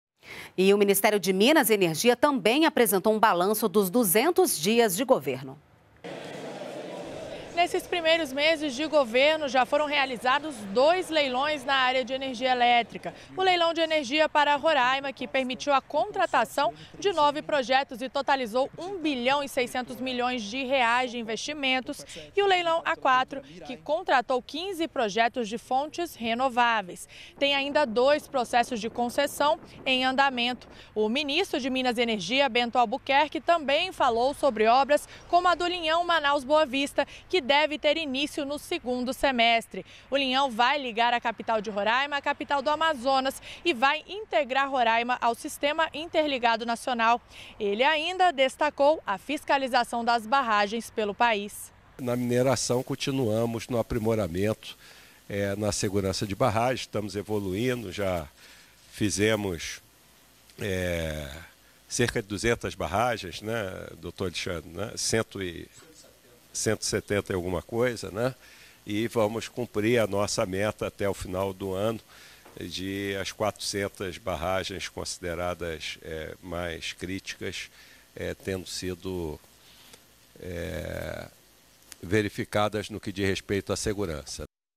Minas e Energia faz balanço de 200 dias de trabalho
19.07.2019 - O Ministério de Minas e Energia apresentou um balanço dos primeiros 200 dias de governo, como a realização de leilões na área de energia elétrica.